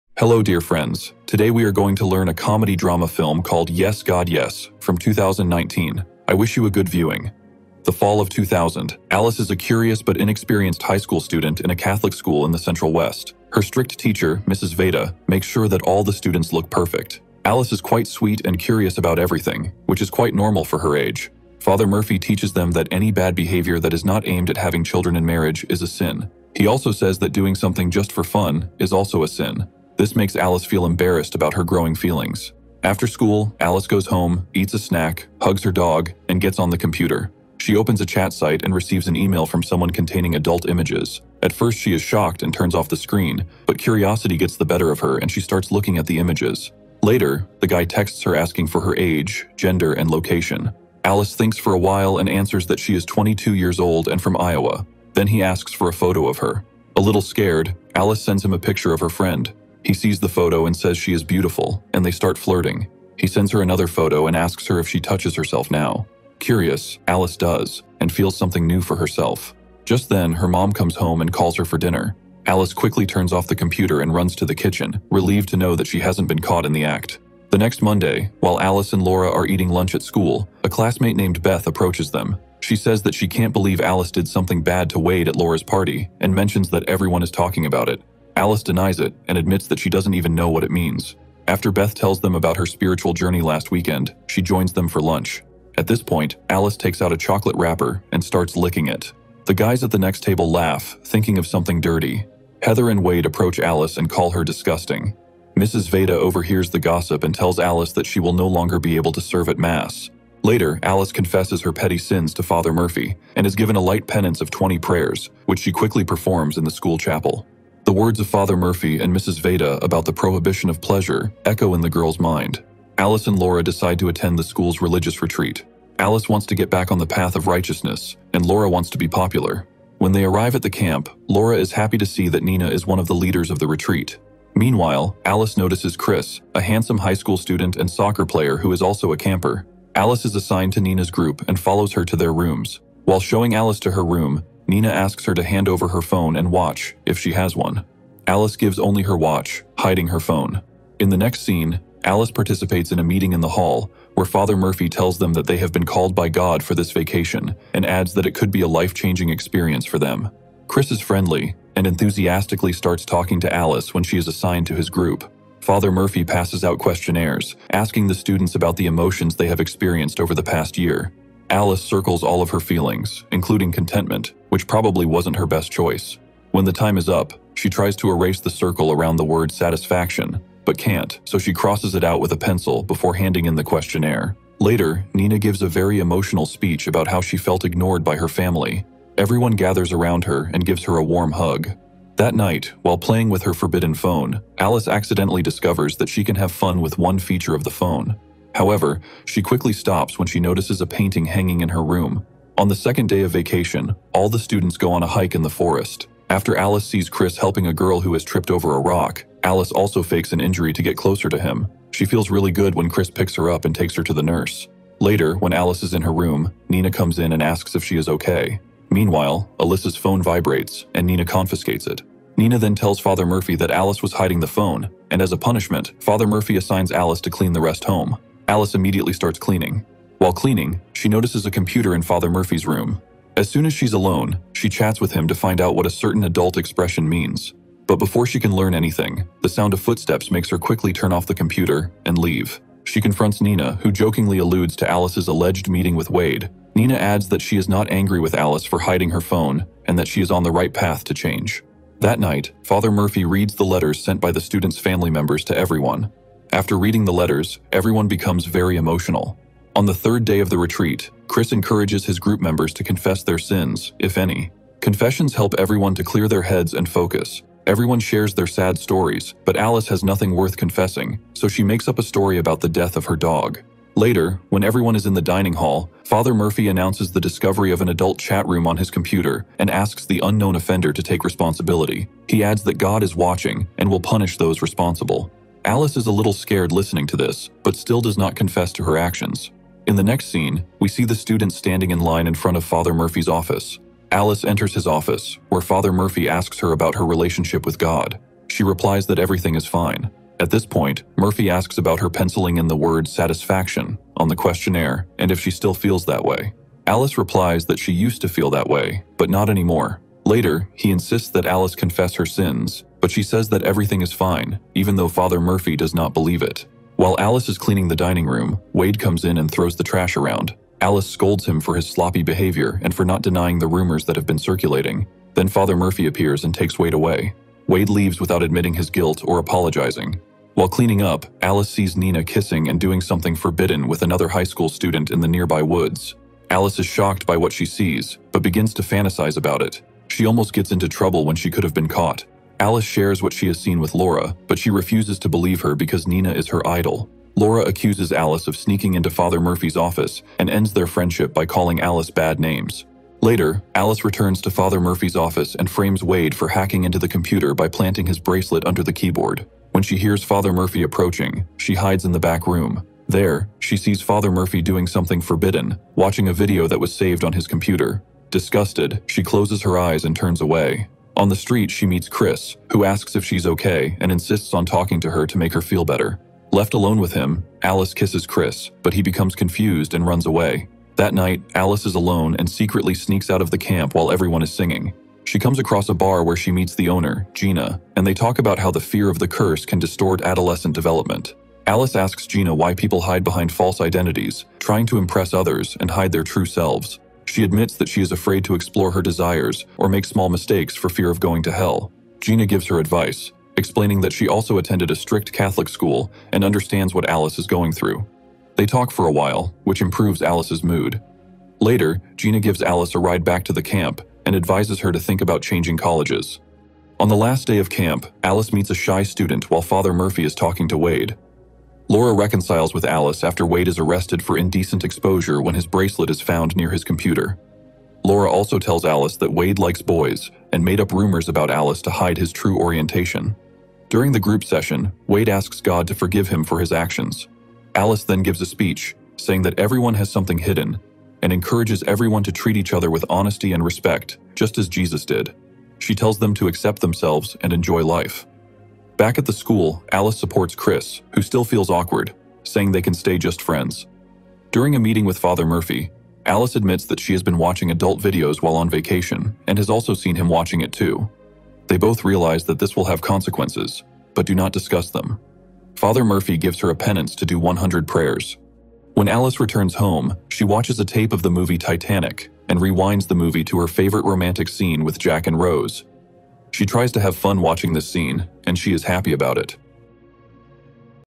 A Girl Was Forbidden To Communicate With Boys So She Solved The Problem Herself
The film tells the story of a girl who was forbidden to communicate with boys, and she didn't really want to. But one day she was alone in her room in front of the computer and decided to try something new.